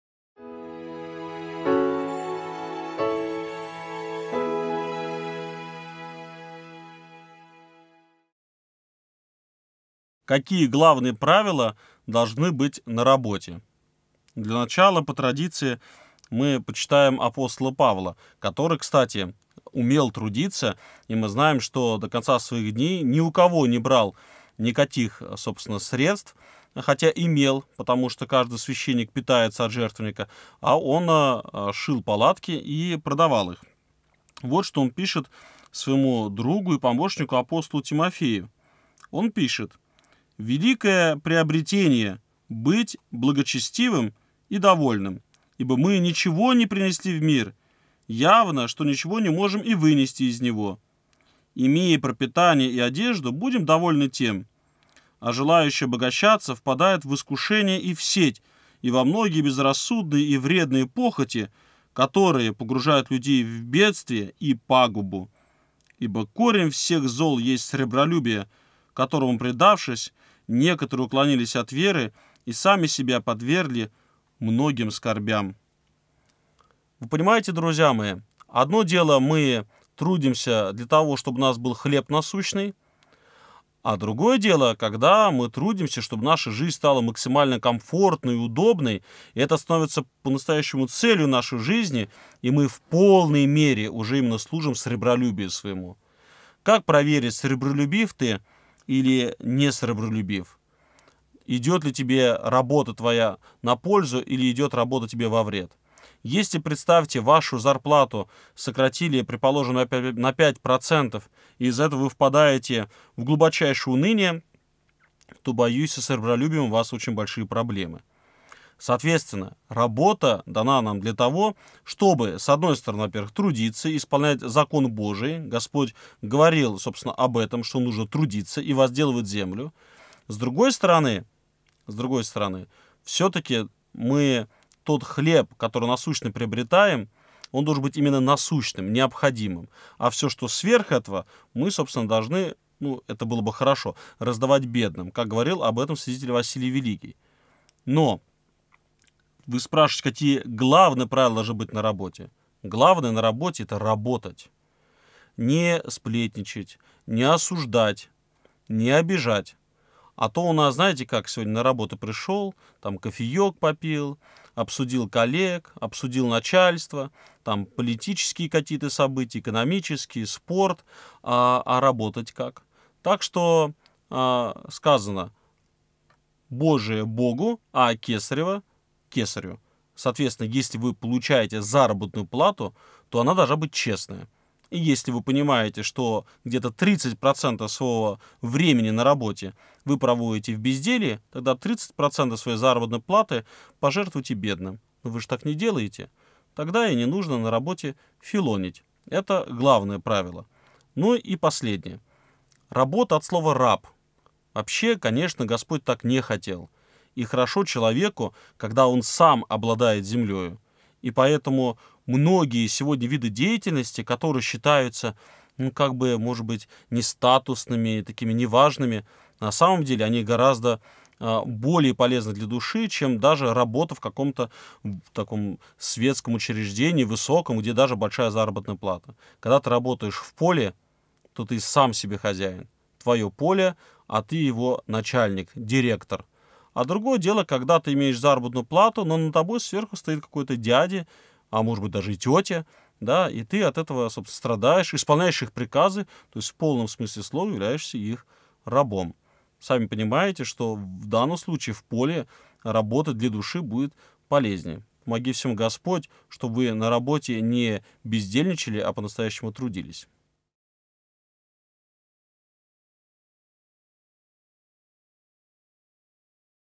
Главные правила на работе. Библия отвечает. Священник Павел Островский
Бог заповедал трудиться всему человечеству, начиная с Адама. Какие же правила должны быть на работе?  Обратимся к примеру Апостола Павла, который был трудолюбив и сам добывал себе пропитание.
О. Павел Островский, настоятель Никольского храма г. Красногорска, даёт ответ на жизненные вопросы современных людей на основании Слова Божия.

📜 Читать 1-ое послание к Тимофею ап. Павла 6 глава 6 стих (с толкованиями)